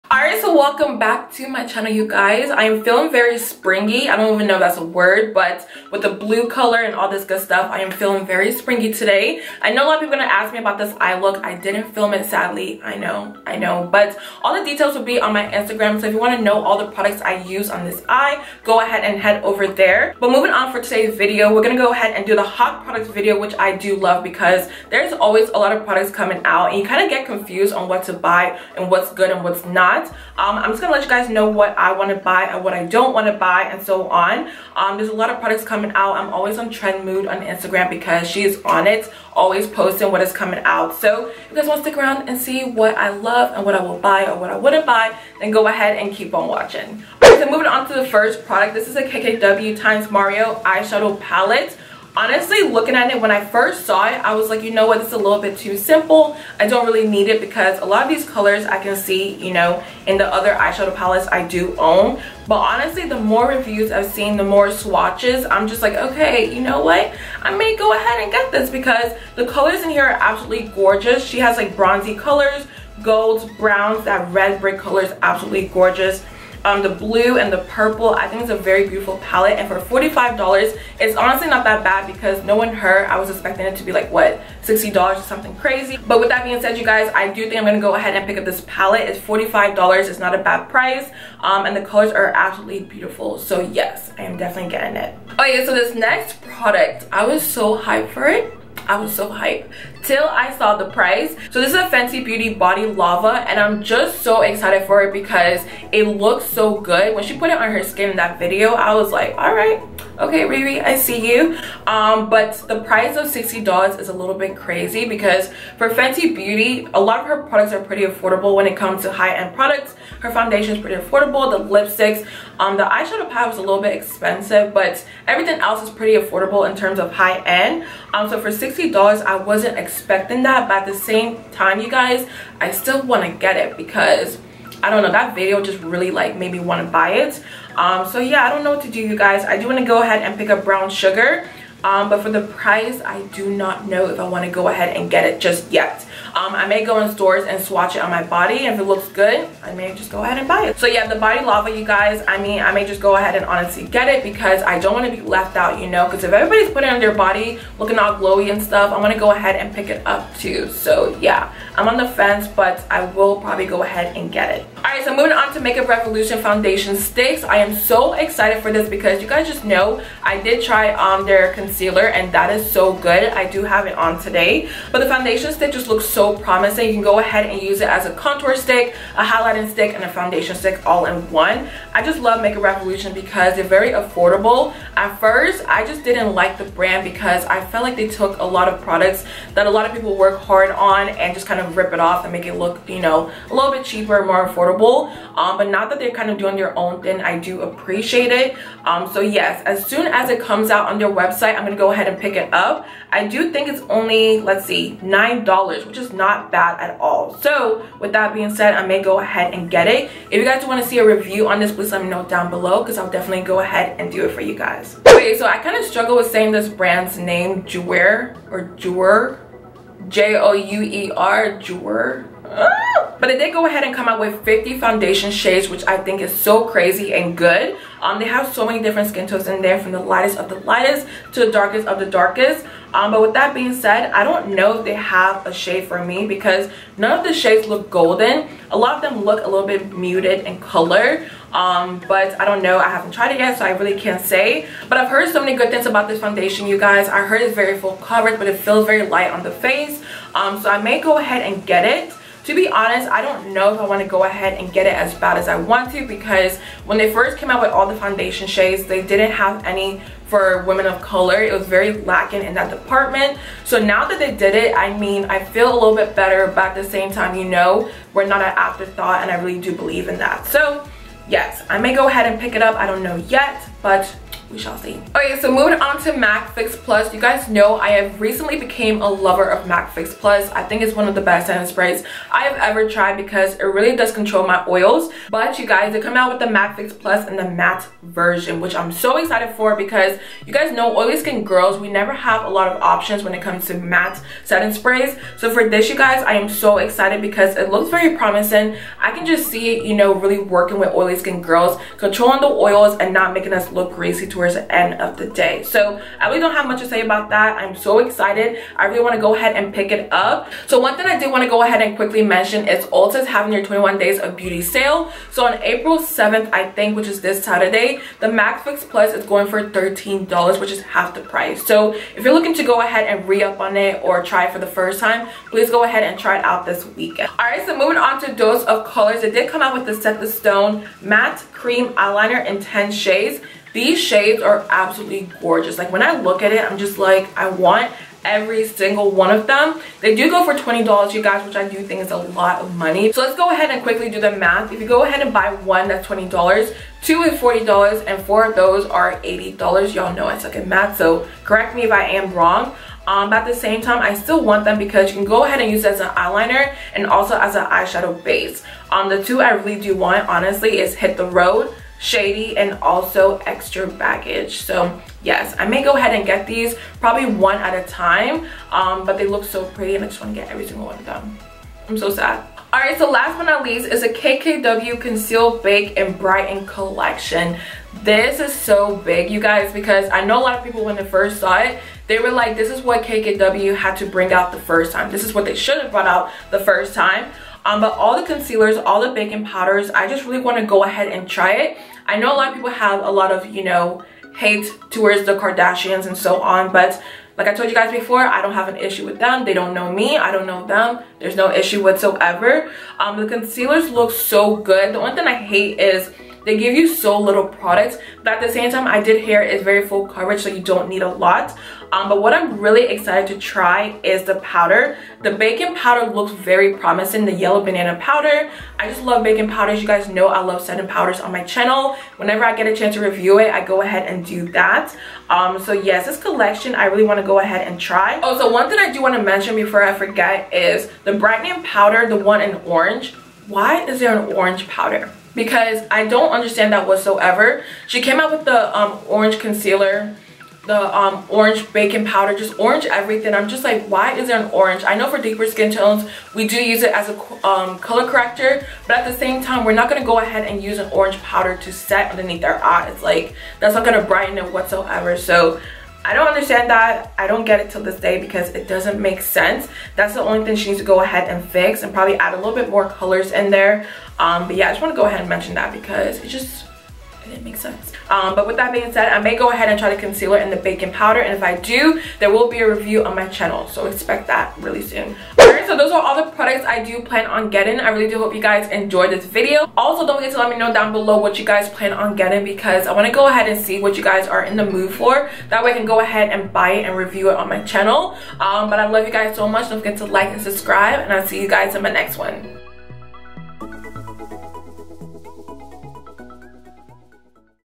Hot New Products! l Part II
Open..♡

KKW X Mario Eyeshadow Palette
Fenty Beauty Body Lava
Makeup Revolution Fast Base Foundation Sticks
Jouer High Coverage Creme Foundation
Mac Matte Fix Plus
Dose of Colors Set The Stone Cream Matte Eyeliner
KKW Beauty Conceal, Bake, Brighten Kit